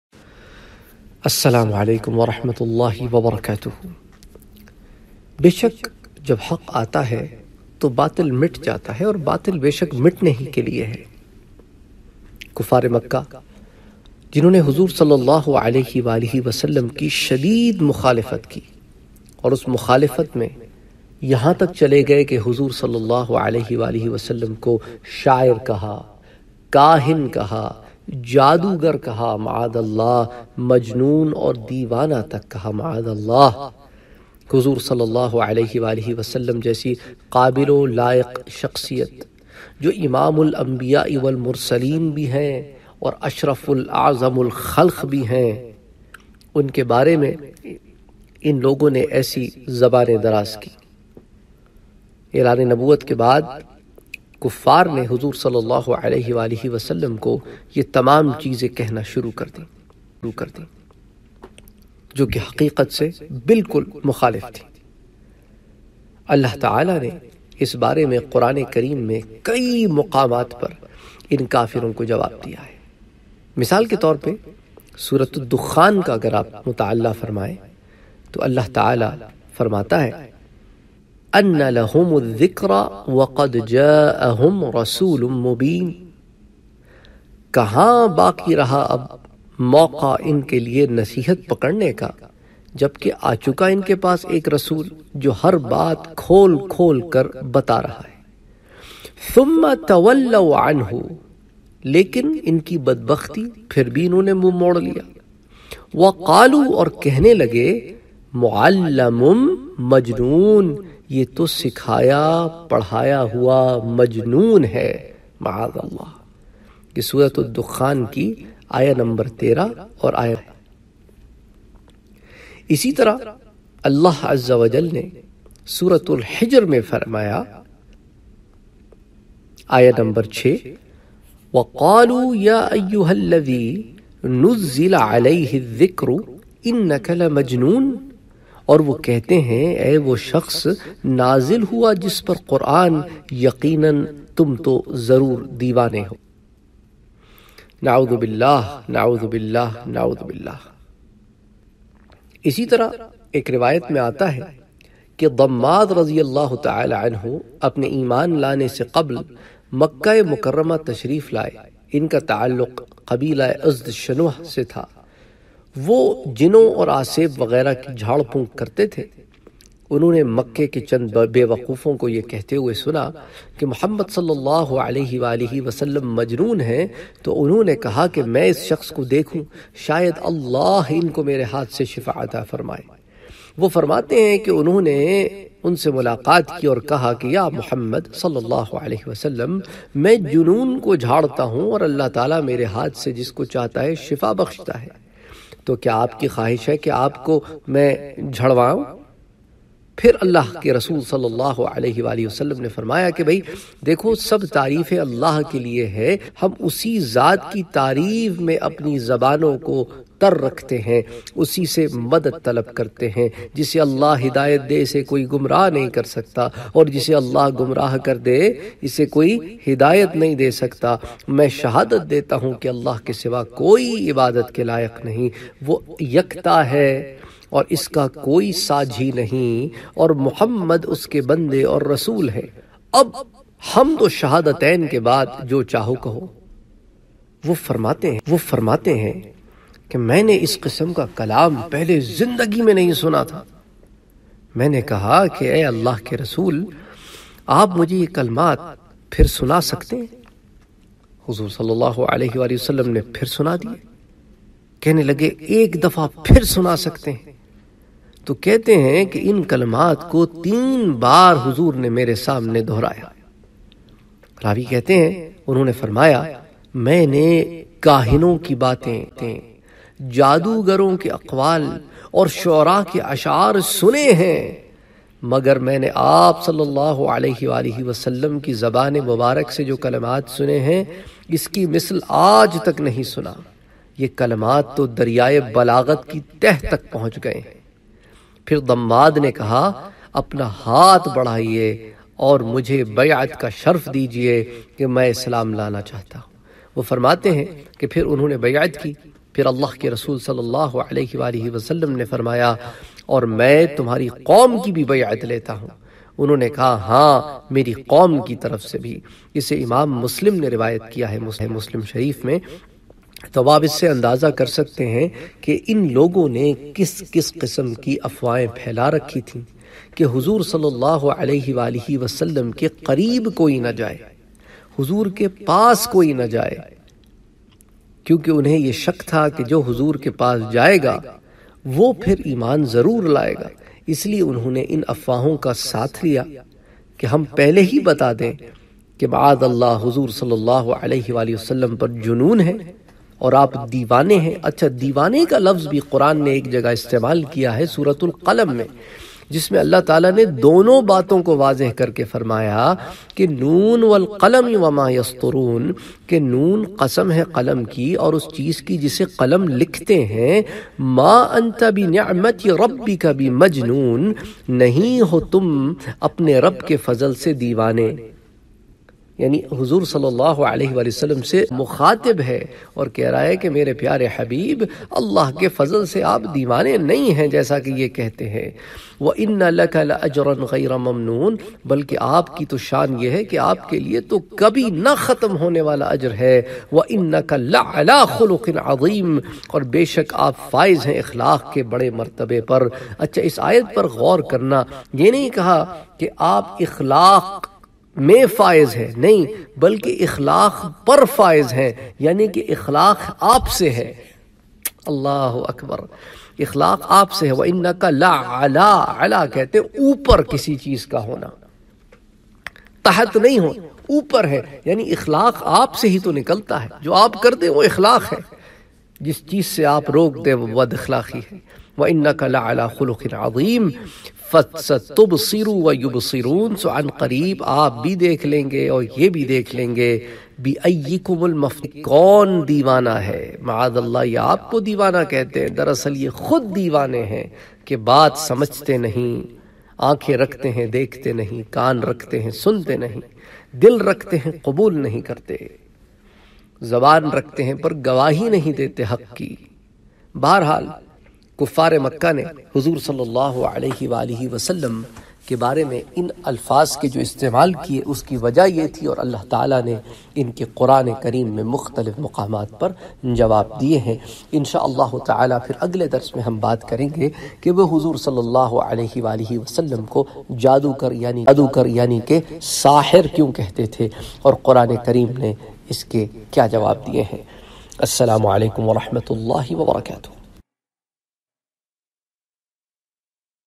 Prophet Muhammad SAWS ko Kafir Dewana kyun kehte thay
Prophet Muhammad SAWS ko Kafir Majnoon kyun kehte thay
Quran ka jawab